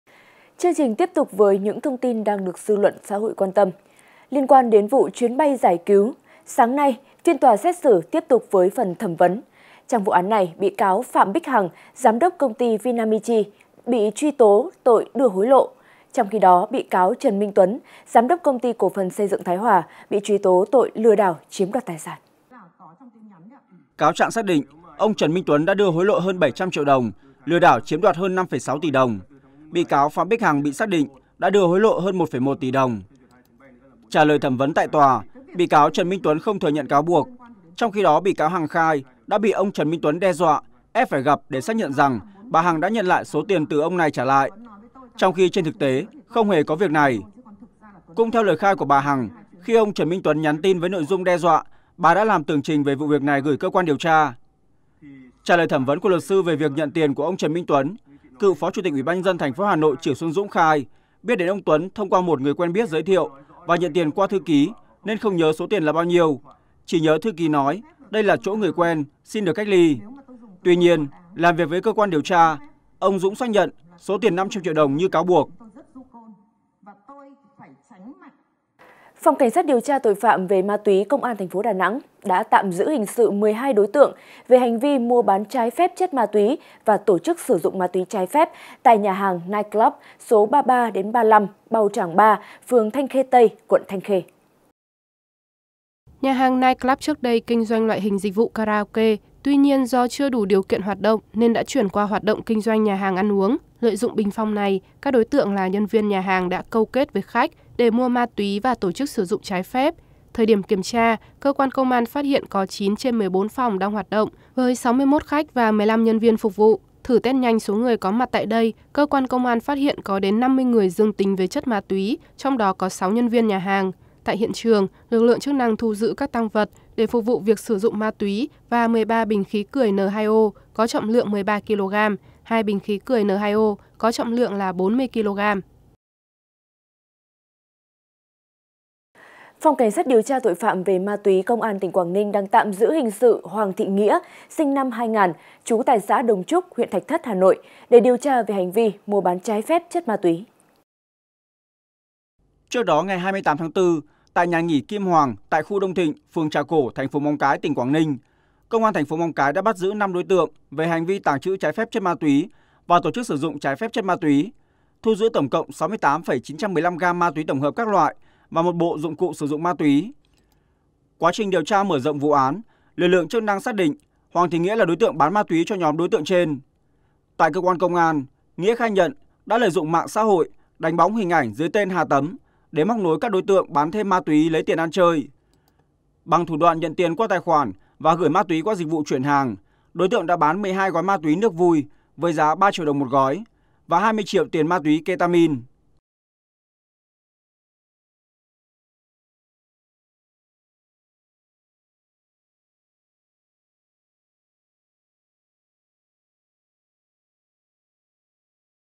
Tin Nóng: Cựu Phó Chủ tịch Hà Nội khai nhận tiền qua thư ký, không nhớ số tiền bao nhiêu | VOV
Cáo trạng xác định, ông Trần Minh Tuấn đã đưa hối lộ hơn 700 triệu đồng, lừa đảo chiếm đoạt hơn 5,6 tỷ đồng. Bị cáo Phạm Bích Hằng bị xác định đã đưa hối lộ hơn 1,1 tỷ đồng.
Trả lời thẩm vấn tại tòa, bị cáo Trần Minh Tuấn không thừa nhận cáo buộc. Trong khi đó, bị cáo Hằng khai đã bị ông Trần Minh Tuấn đe dọa, ép phải gặp để xác nhận rằng bà Hằng đã nhận lại số tiền từ ông này trả lại. Trong khi trên thực tế không hề có việc này. Cũng theo lời khai của bà Hằng, khi ông Trần Minh Tuấn nhắn tin với nội dung đe dọa, bà đã làm tường trình về việc này gửi CQĐT.